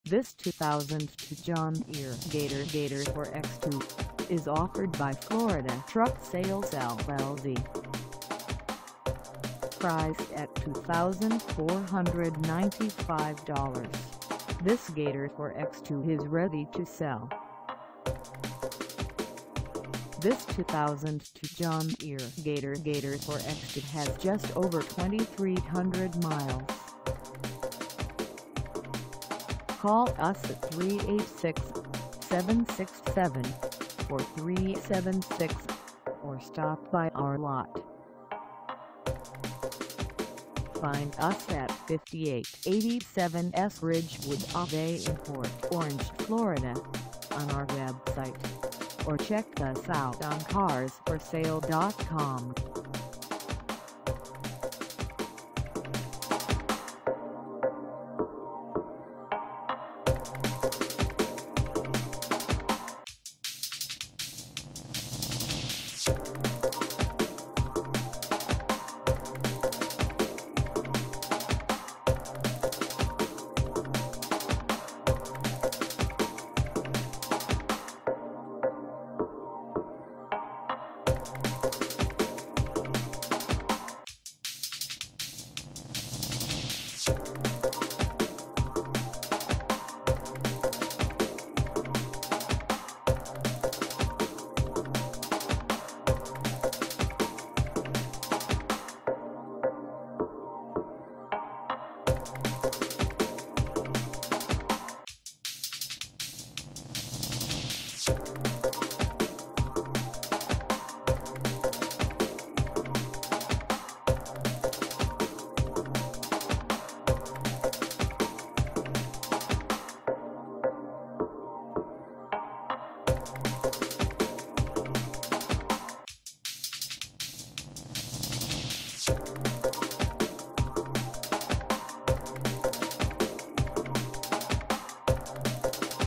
2002 John Deere Gator Gator 4X2 - for sale in PORT ORANGE,
FLORIDA TRUCK SALES LLC

Come test drive this 2002 John Deere Gator Gator 4X2  for sale in PORT ORANGE, FL.